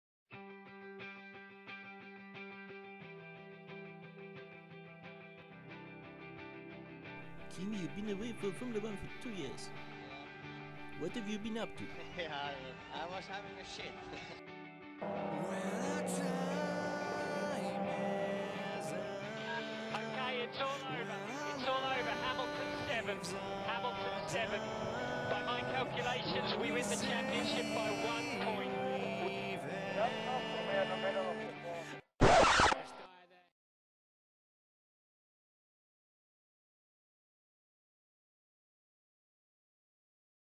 Welcome back to F1, Kimi!
Yep, it's official... Okay, it isn't. But soon to be... I hope.

Sorry about the low volume. Realised I had been using my very loud headphones when editing when it was too late.

The overall quality of this one is lacking a bit. Just a quick project.